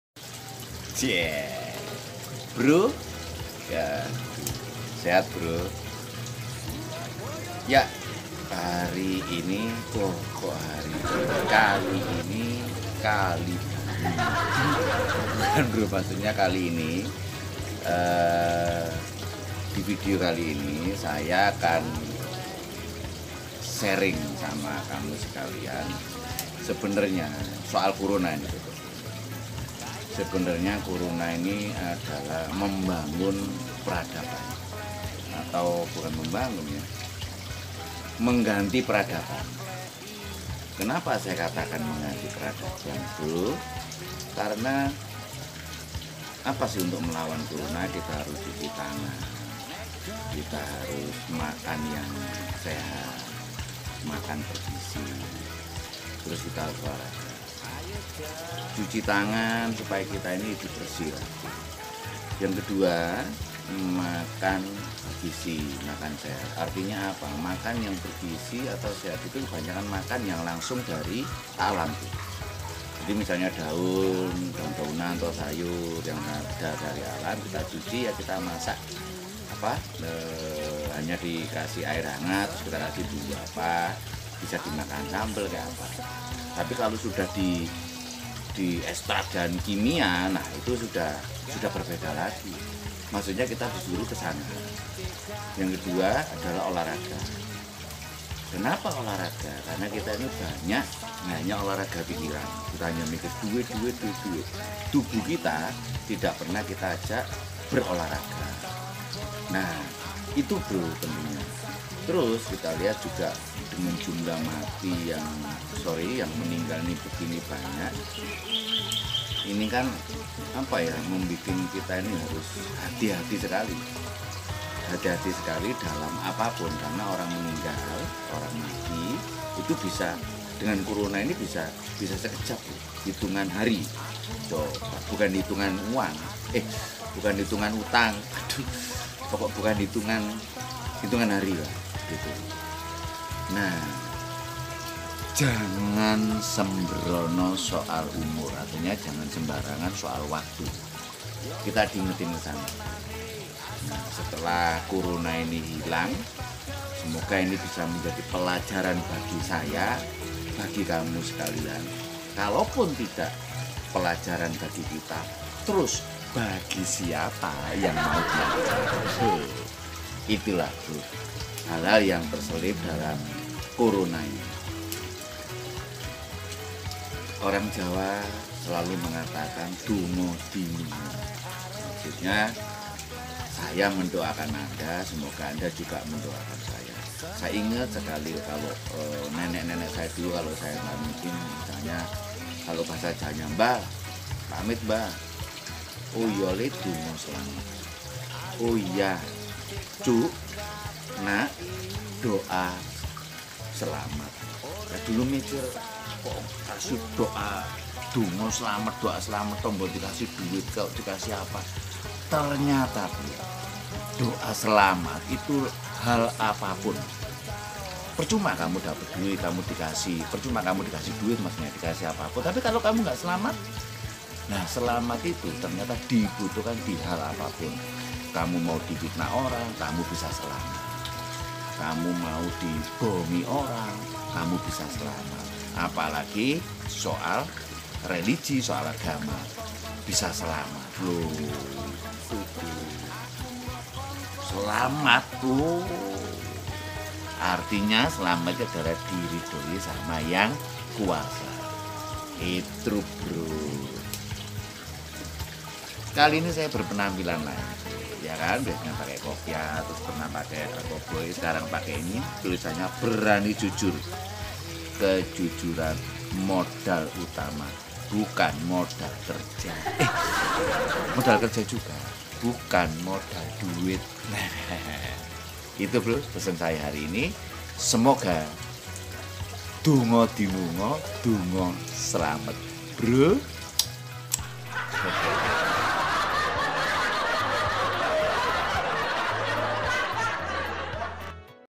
Corona Mengganti PERADABAN YANG RUSAK 1 - Cak Suro
Ngakak trs-trs ngakak HIKMAH DENGAN ADANYA CORONA Ngakak trs-trs Ngakak sy hanyalah seorang penghibur beŕharap setelah nonton vidio2 sy paling tdk bisa membuat kamu senyum.. swn